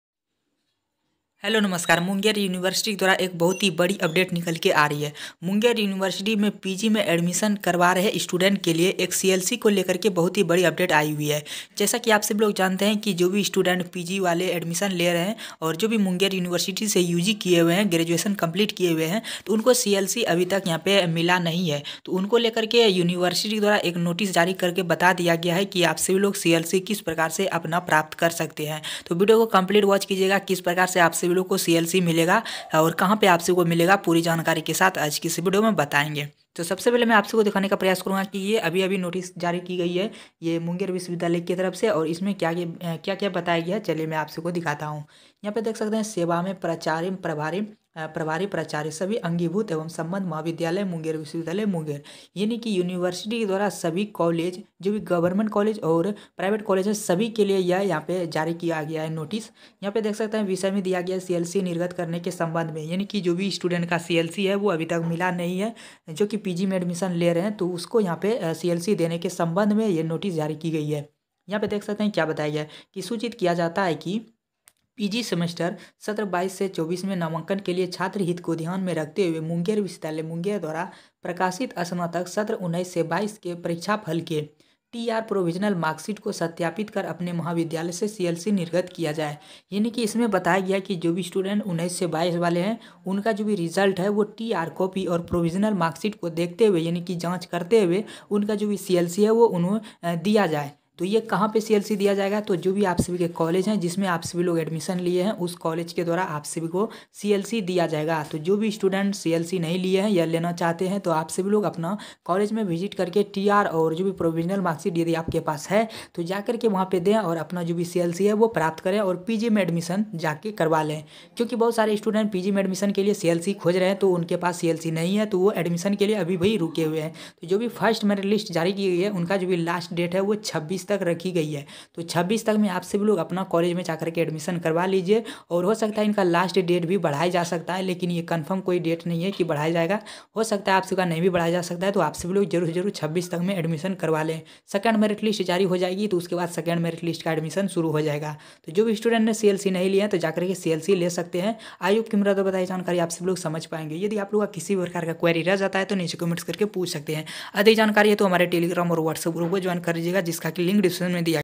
Munger University PG Admission | सत्र (2019-22) वाले को ऐसे मिलेगा | CLC Official Notice हुआ जारी
🙏THANK YOU 🙏                आप लोग ऐसे ही सपोर्ट करते रहें।                      GULSHAN STUDY